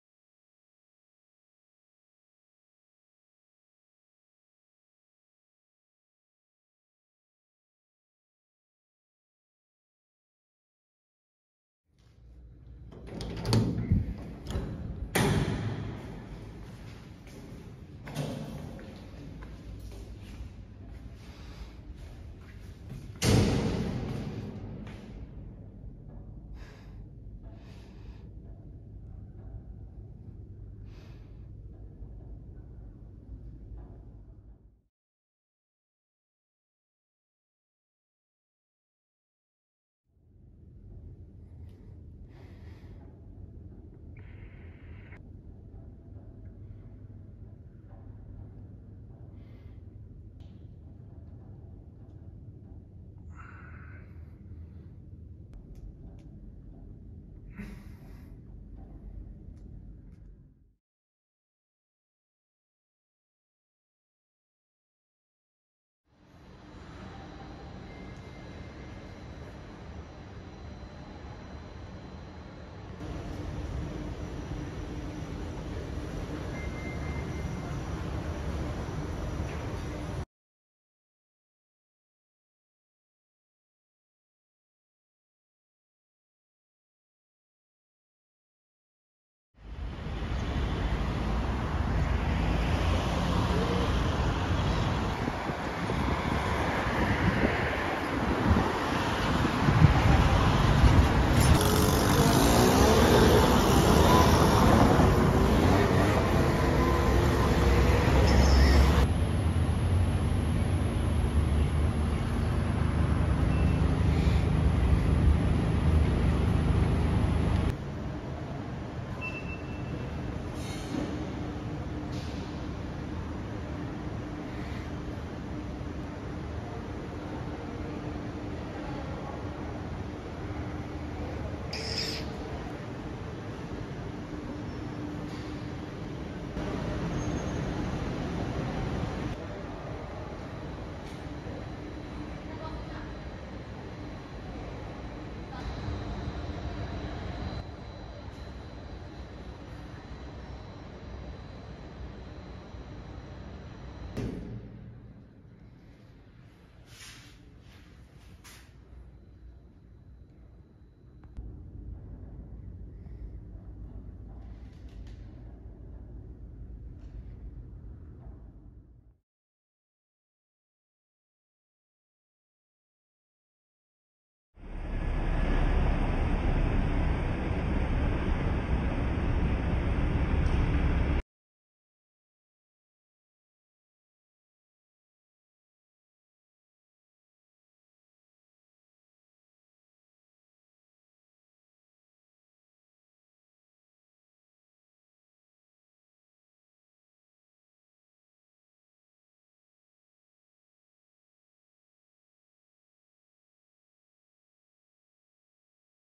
The Adelphi Musollah - Detailed Information (Near City Hall MRT)
TIMECODES⏳
00:00 - Intro
00:08 - Musollah’s Condition (Shared)
00:36 - What does the Musollah provide?
01:02 - Nearest Toilet to the Musollah
01:27 - Route from City Hall MRT
02:51 - Carpark for Vehicle User
03:09 - Outro

SHORT SUMMARY 📖
- Male and Female shared
- Wudhu area at the Nearest Toilet
- Prayer mat provided
- Telekong provided
- Sarong provided
- Slippers provided

NEAREST MRT 🚝
- City Hall

NEAREST CARPARK 🚘
- Adelphi Carpark (1 coleman street)